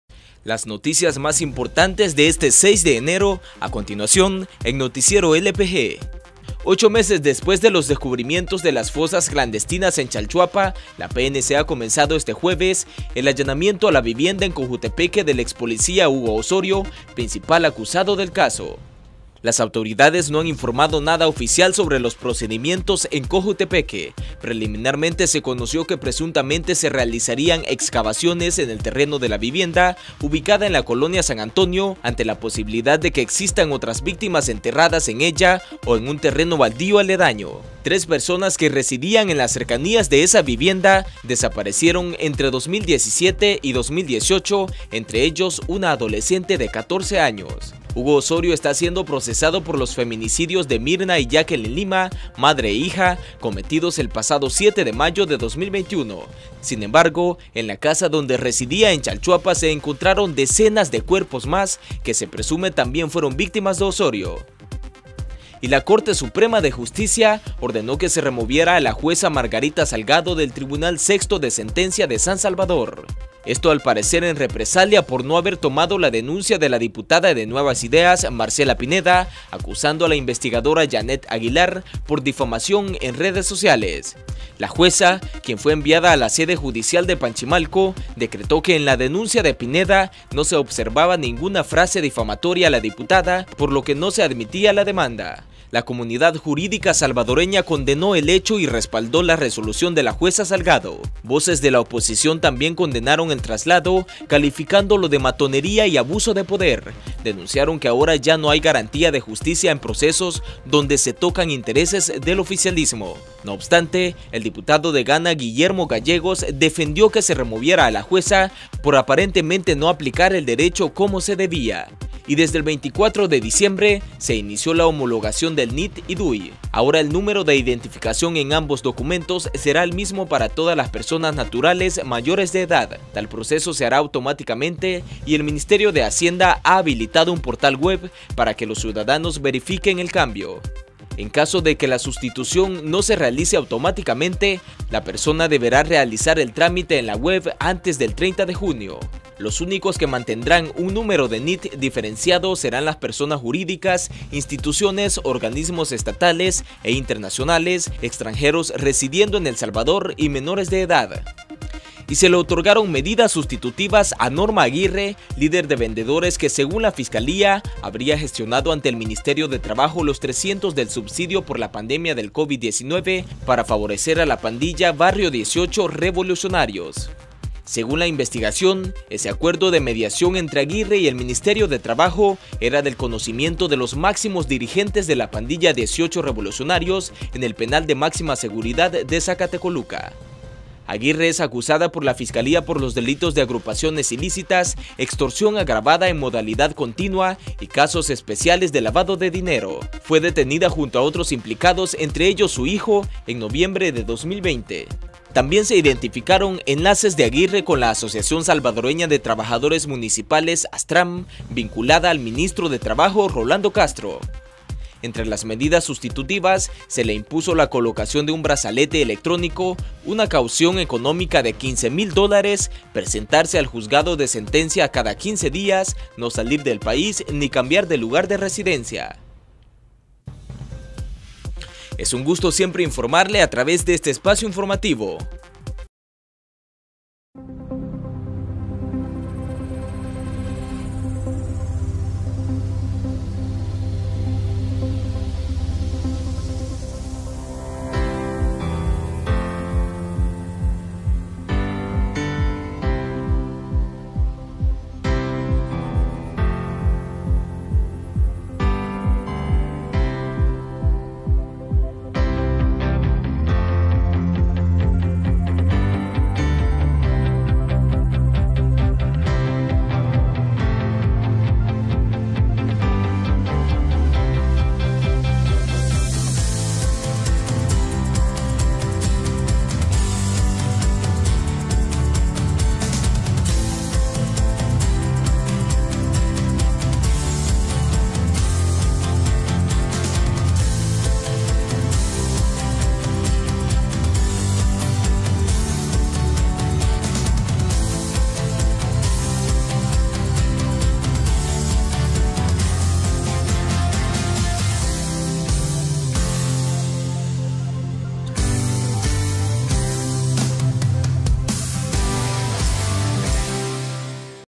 Noticiero LPG 6 de enero: Realizan allanamiento de casa del expolicía Hugo Osorio en Cojutepeque
Ocho meses después de los descubrimientos de las fosas clandestinas en Chalchuapa, la PNC ha comenzado este jueves el allanamiento a la vivienda en Cojutepeque del expolicía Hugo Osorio, principal acusado del caso.

¡Visítenos!